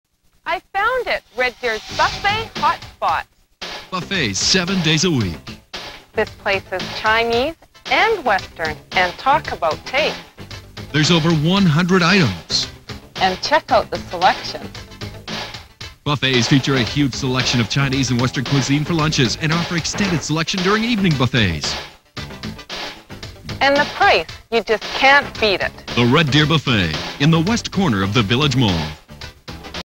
Red Deer Buffet commercial from 1997
Pulled from VHS Tape of November 18th 1997 broadcast of RDTV (Red Deer)